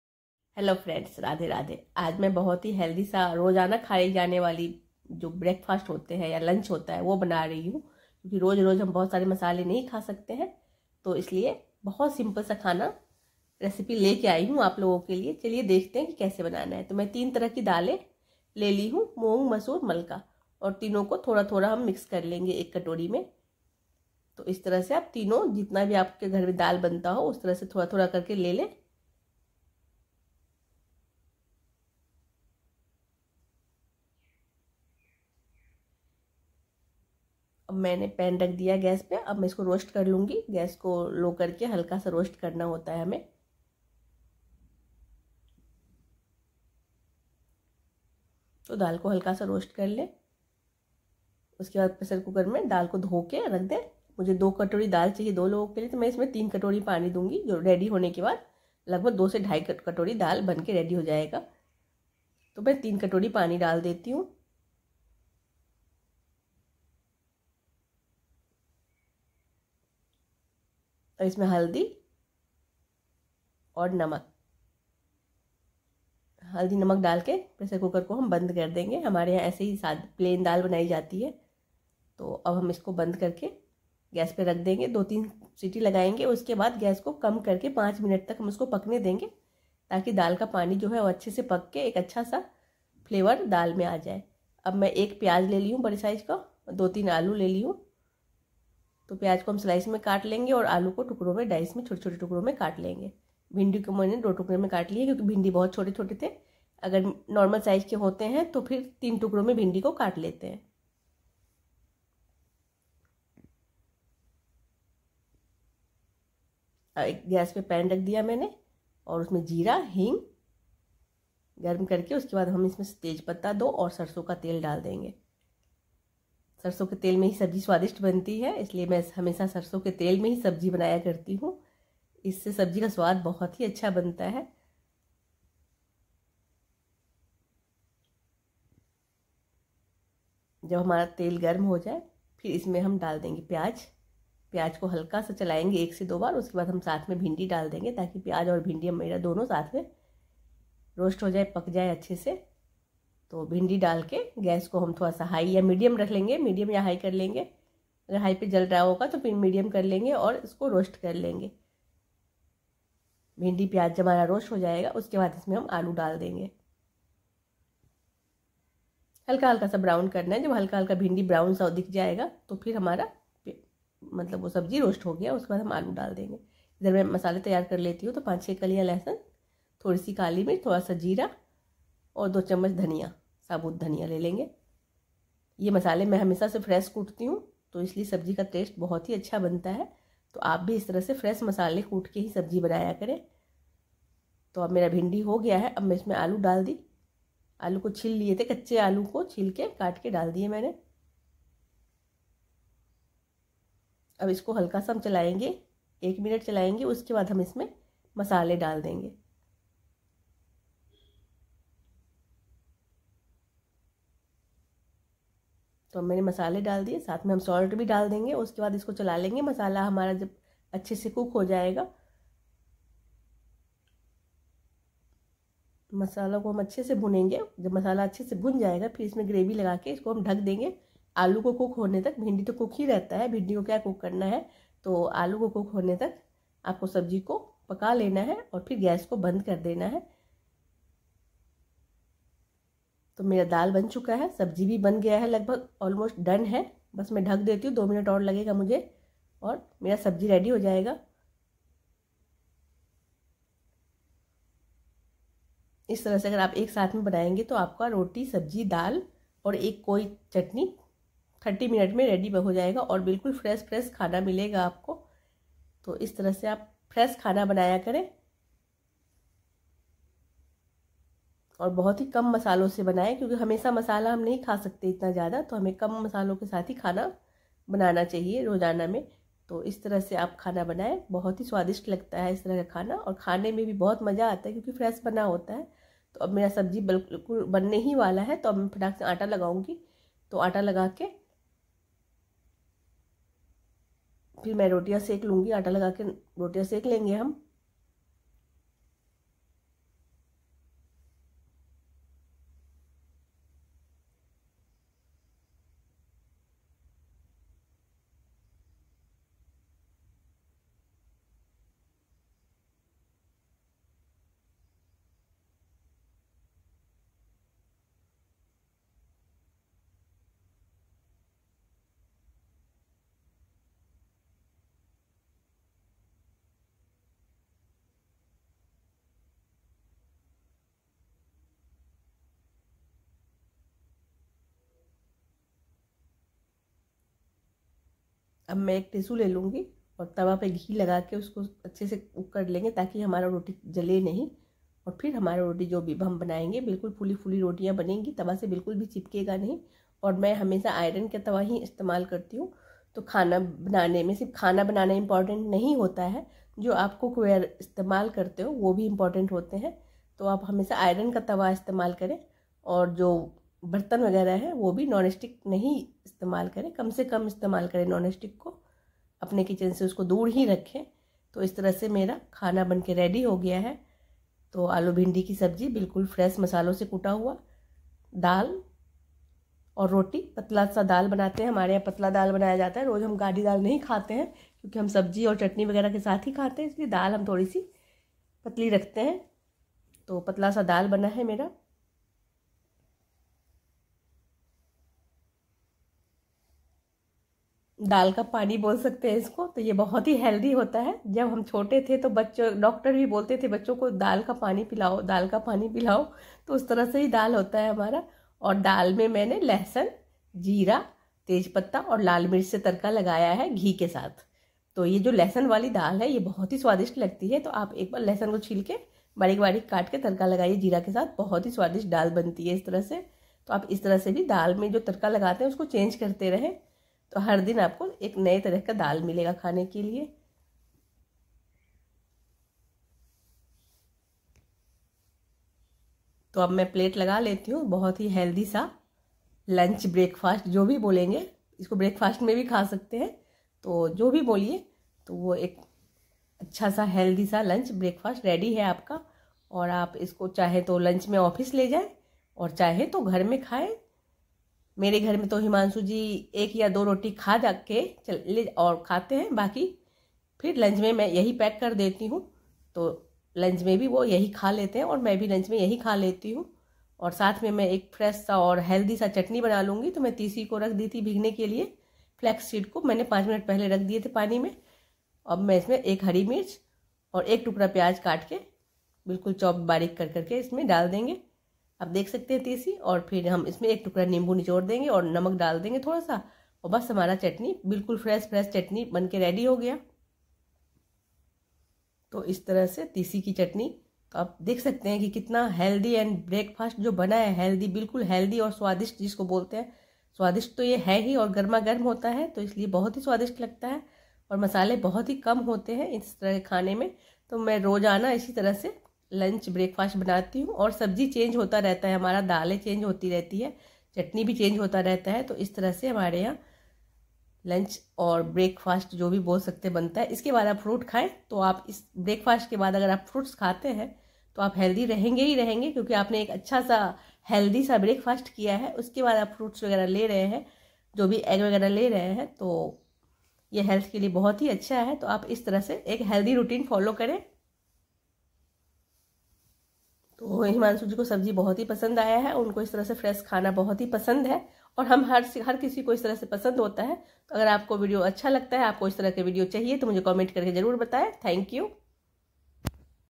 Indian lunch recipe 😋 रोटी ,सब्ज़ी, लहसुन वाली दाल और Flaxseed ki Chatney .
Indian lunch recipe 😋 रोटी ,सब्ज़ी, लहसुन वाली दाल और Flaxseed Chatney . बिना मसालों वाली भिन्डी ।